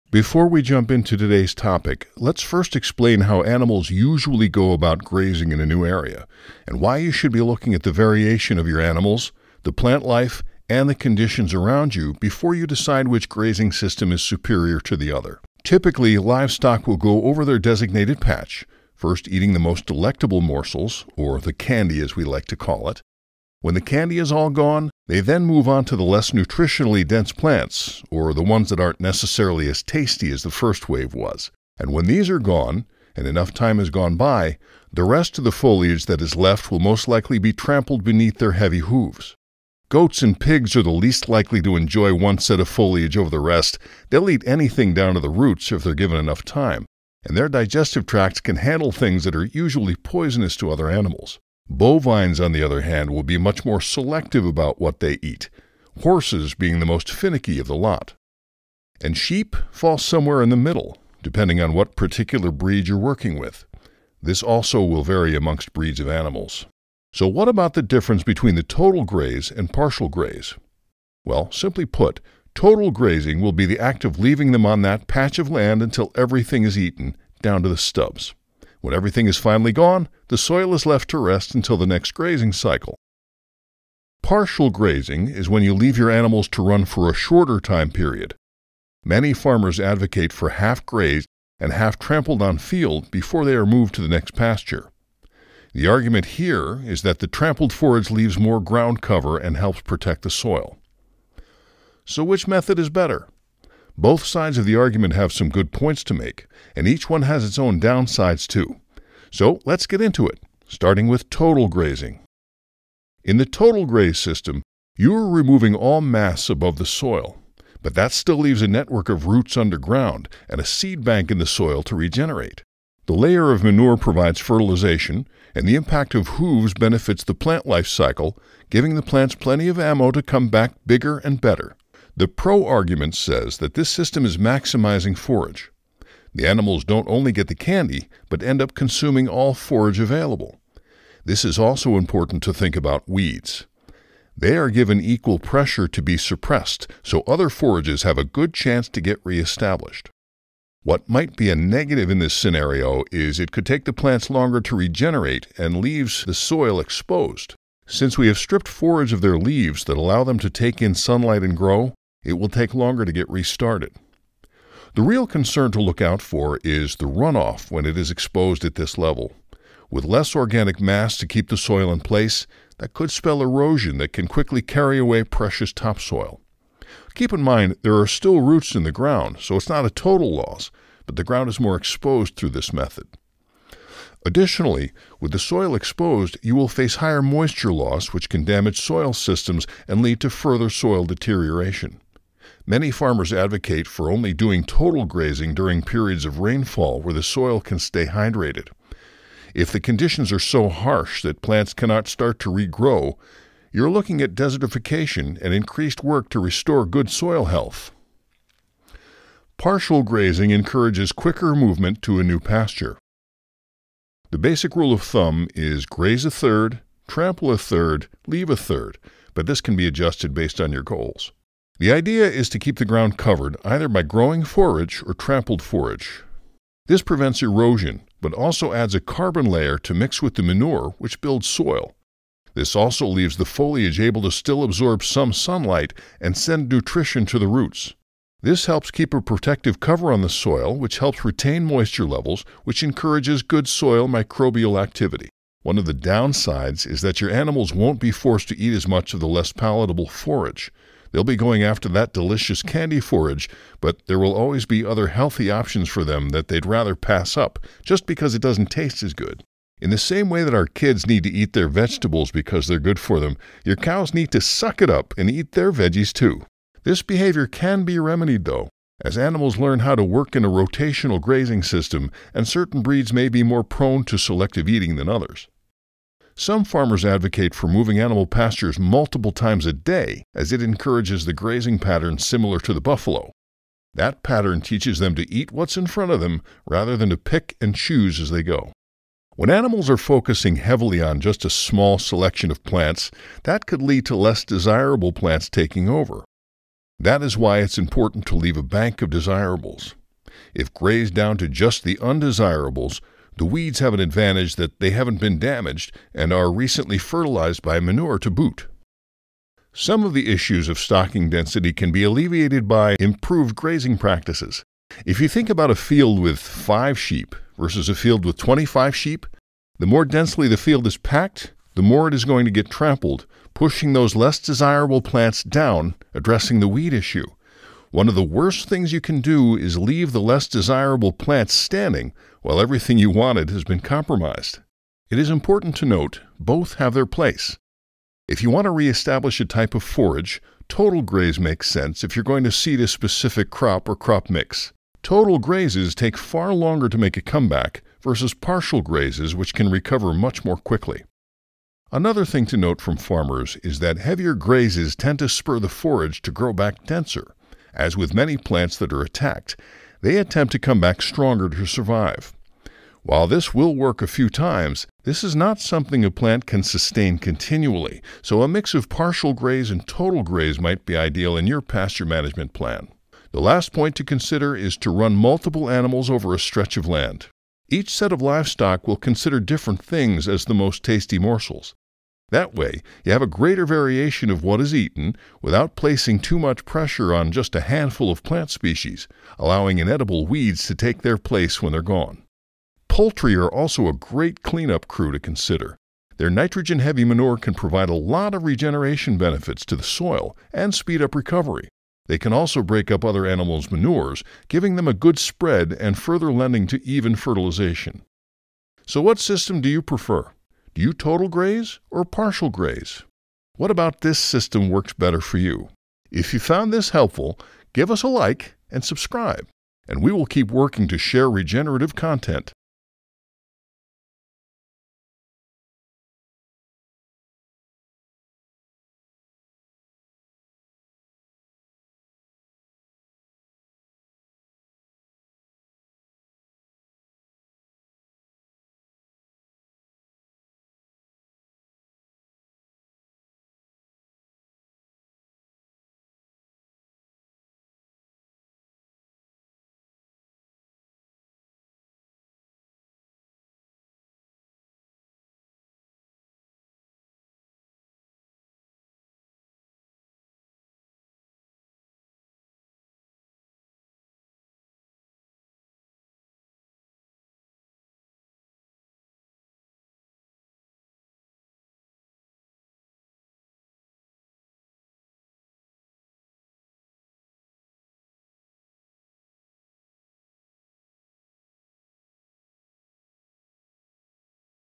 Total vs Partial Grazing: Which is better for Rotational Grazing?
In this video, we'll explore the debate between total and partial grazing in rotational grazing systems. Find out which method is more effective for maximizing pasture productivity and livestock health!